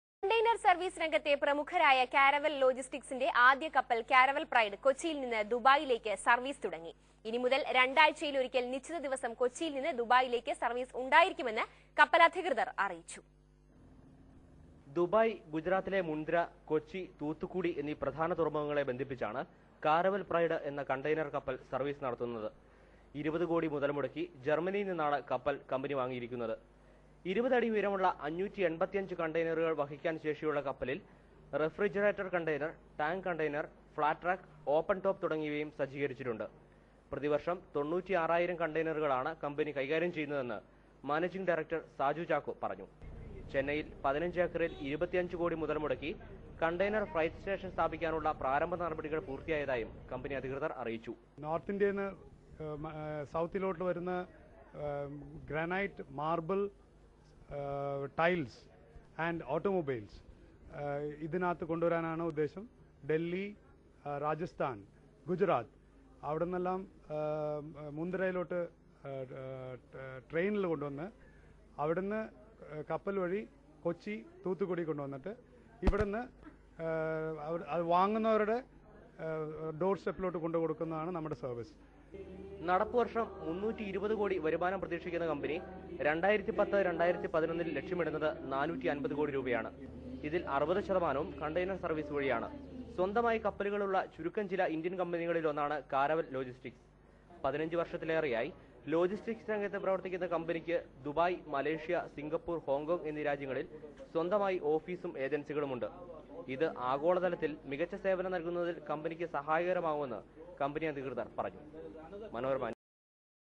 Caravel Pride starts Kochi service
Container vessel Caravel Pride now connects Kochi with Dubai